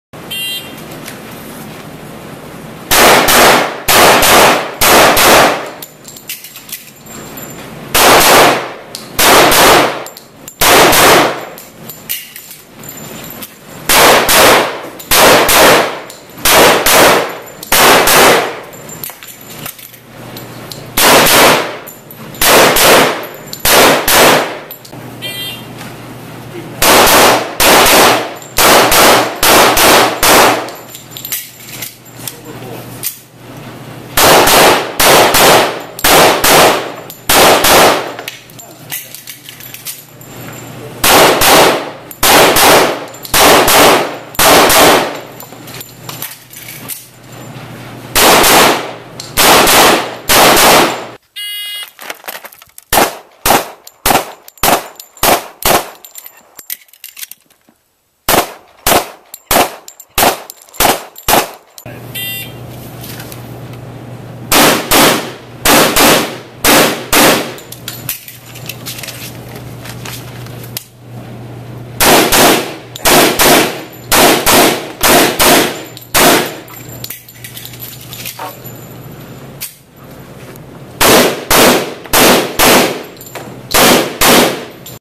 Derek Wallace @ MSC 1-18-12
First match back after a long break.  I switched to single stack and it looks like it is going to be a ton of fun.  Had some trouble finding the sight off the draw after switching from a Glock and I need to get used to slide lock reloads now that I am short two more bullets.  According to CmCalc I managed a 62.24% on Melody Line (99-08), and I am pretty happy with that.